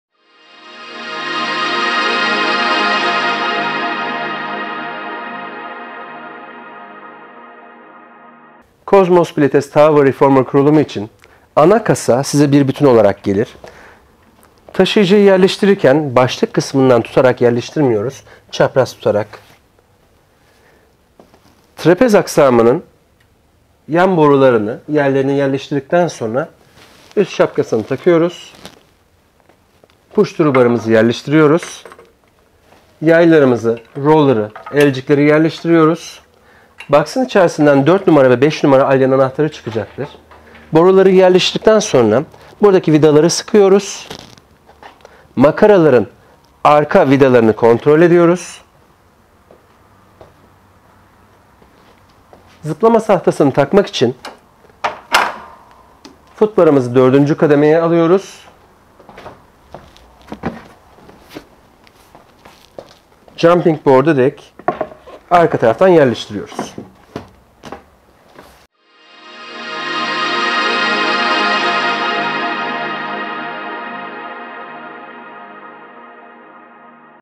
Cosmos Tower Reformer Kurulumu
Cosmos Tower Reformer

Ürün Detayı :
• 1. Sınıf masif kayından imal edilmiş gövde
• 5 Farklı dirençteki yay sistemi
• Sessize yakın sürüş ile 8 adet tekerlekli taşıyıcı araba
• 4 Farklı açıdan ayarlanabilen footbar sistemi
• 3 Kademeli stoper kaydırmaz ve antibakteriyel deri döşeme
• 3 Farklı şekilde kolay değiştirilebilir gearbar
• 3 Farklı derecede ayarlanabilen başlık
• Geliştirilmiş makara sistemi
• 3 mm çelik trapez aksamı, isteğe bağlı olarak krom kaplama
veya siyah static boya seçenekeleri

Aksesuarlar :

• 1 Çift Strap (Elcik) , 1 Çift Halat
• 1 Adet Jump Board (Zıplama Tahtası)
• 1 Çift Foam Handle (Sünger Elcik)
• 1 Adet Mat Converter
• 1 Adet Pushthrought Bar
• 1 Adet Emniyet Bandı
• 1 Adet Rolldown Bar, 1 Adet Foot Band
• 2 Adet Uzun Sarı Yay, 2 Adet Uzun Mavi Yay
• 2 Adet Sarı Kısa Yay, 2 Adet Mavi Kısa Yay

Ölçüler :

Yükseklik : 40 cm
Uzunluk : 236 cm
En : 70 cm

Deri döşeme rengi ve ahşap rengi dilediğiniz renk ve kombinasyon ile üretilmektedir.